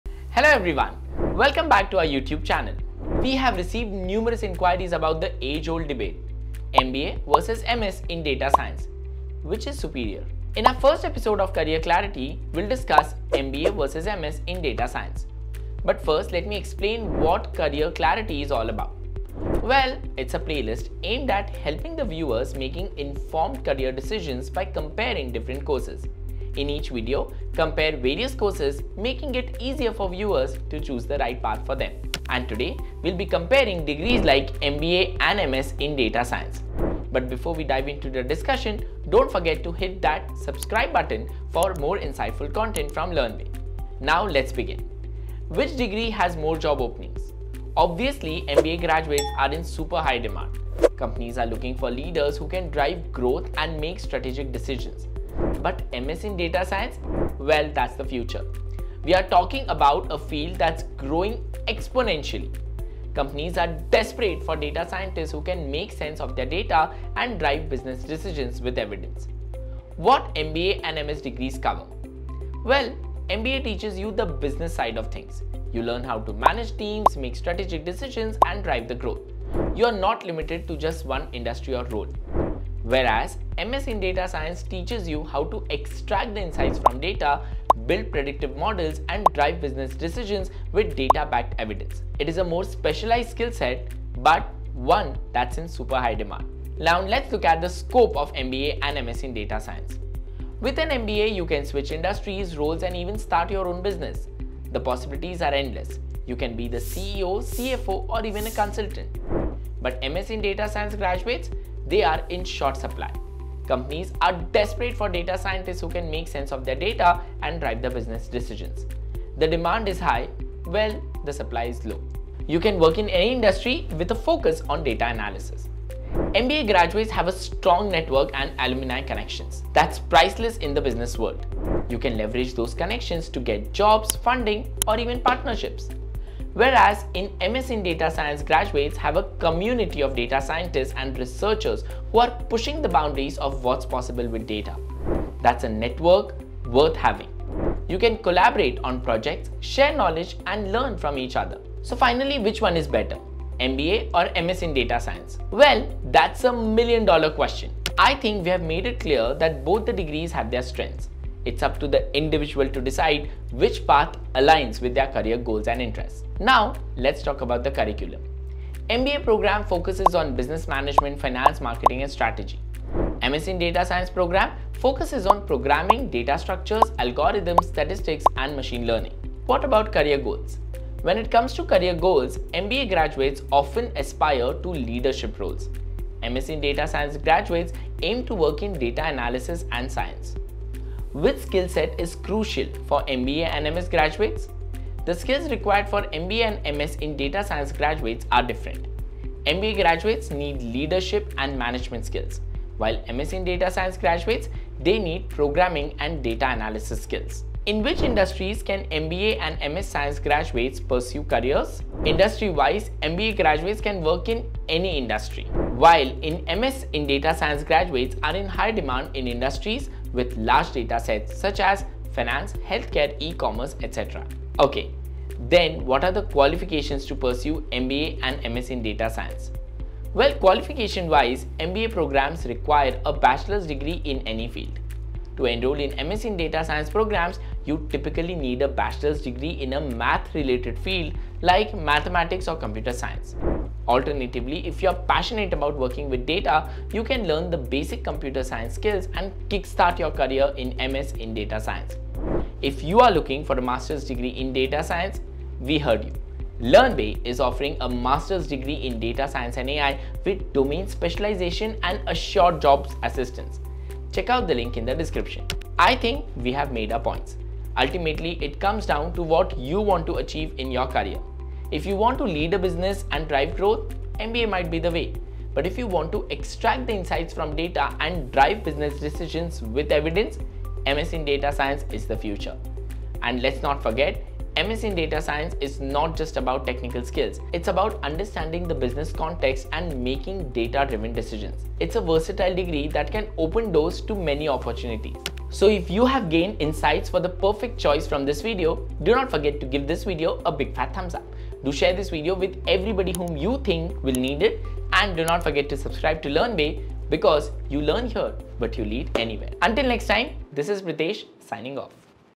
MBA vs MS in Data Science | Which One You Should Choose? | Learnbay
In this video, you can explore different career paths with an MBA in data science vs an MS in data science. Here’s a short comparison between the two to help you decide what’s best for you.